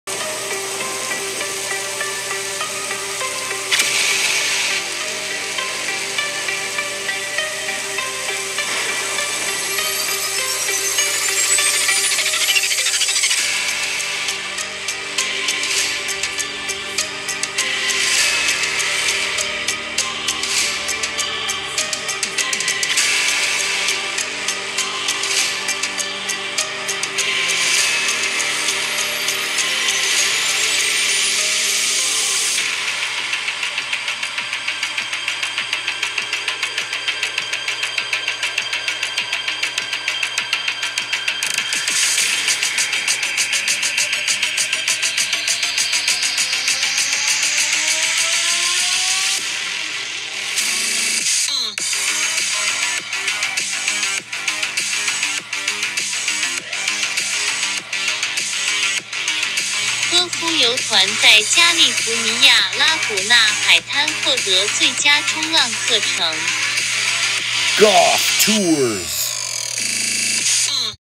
Laguna Beach Surfing Lesson 11-16-20
This is Eddie and Weibo with a 2 to 3 foot swell while taking their very first surfing lesson in Laguna Beach California with Goff Tours 11-16-20, You can tell they both had a great time as they left the beach smiling.